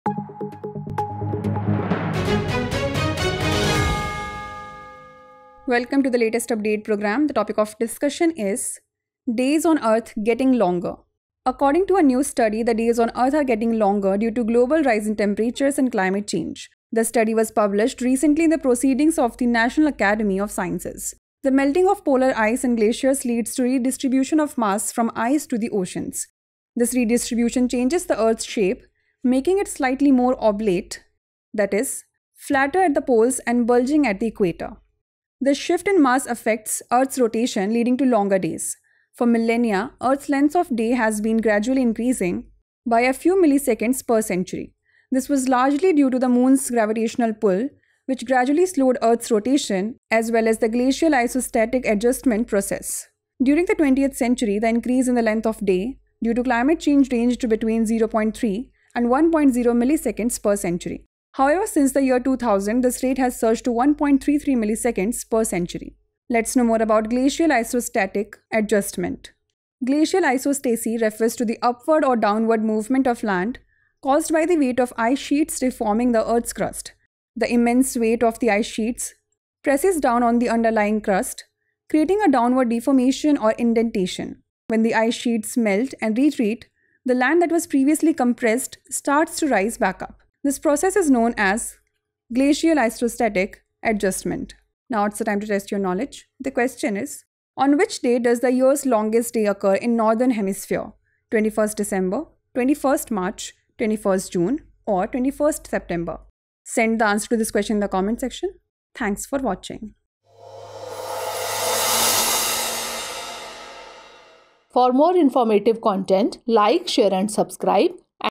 Days On Earth Getting Longer | Climate Change | Latest Update | Drishti IAS English
A new study published in the Proceedings of the National Academy of Sciences reveals that days on Earth are getting longer due to rising global temperatures and climate change. The melting of polar ice and glaciers causes mass redistribution from ice to oceans, altering Earth's shape and rotation. Historically, the length of day increased by 0.3 to 1.0 milliseconds per century, but this has surged to 1.33 milliseconds per century since 2000.